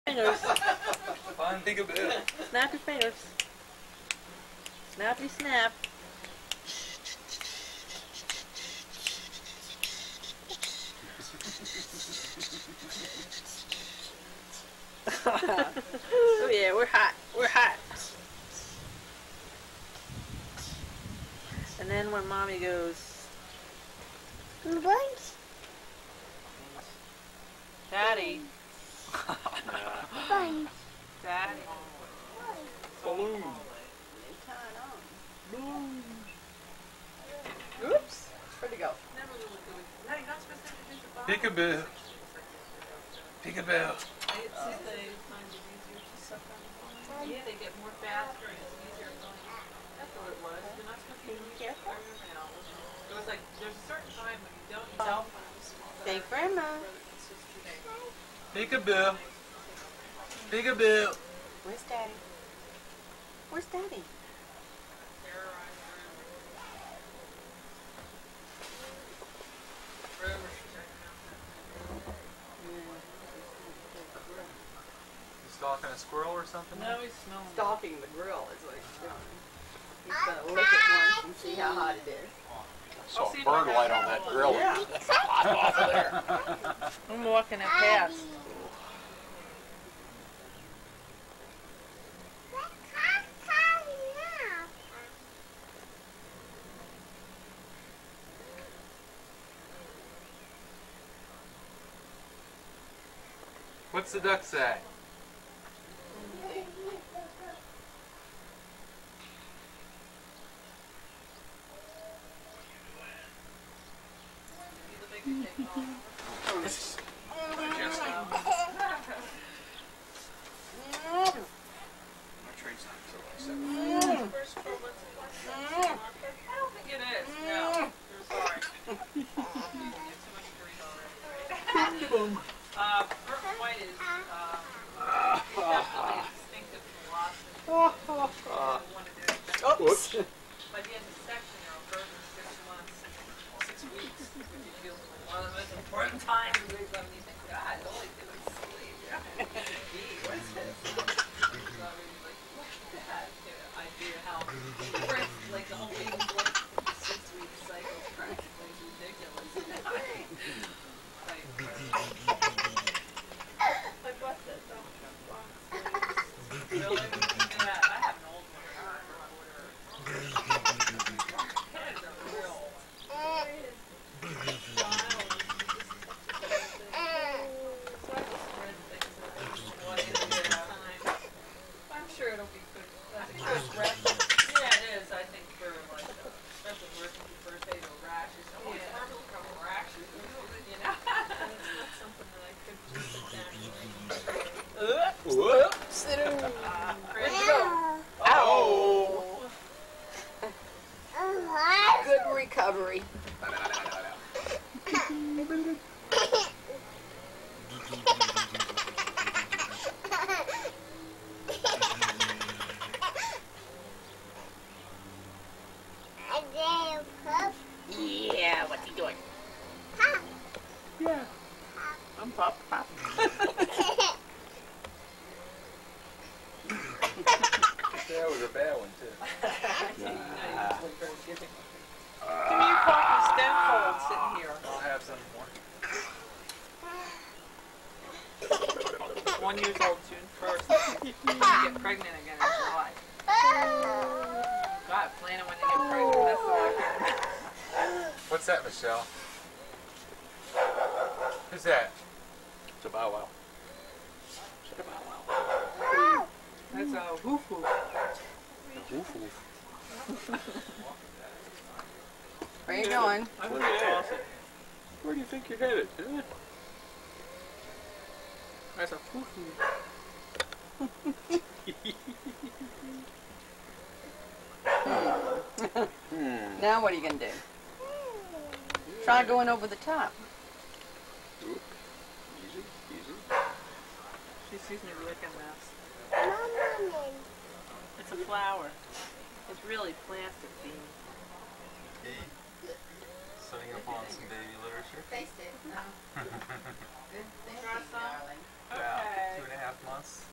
A 1985June 4th , Michelle Ala snapping spring with mom dad sam mar mpg MP4
Michelle, at age 1 1/2, shows her stuff!